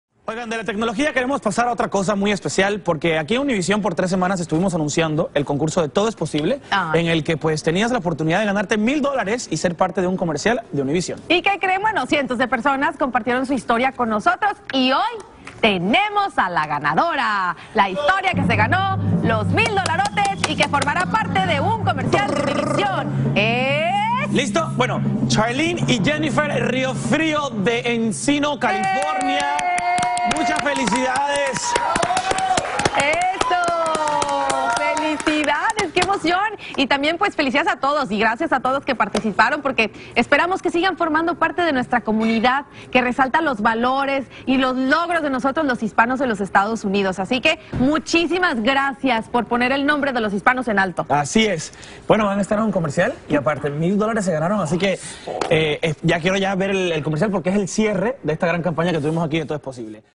Todo Es Posible ¡Sé El Protagonista! Conoce a las ganadoras del concurso
Charlene y Jennifer Riofrio son las ganadoras del concurso de Todo Es Posible ¡Se El Protagonista! Obtuvieron $1000 para continuar su sueño y además serán parte del último comercial del año para Todo Es Posible. No te pierdas su estreno, muy pronto. Visita para conocer mas de esta campaña de Univision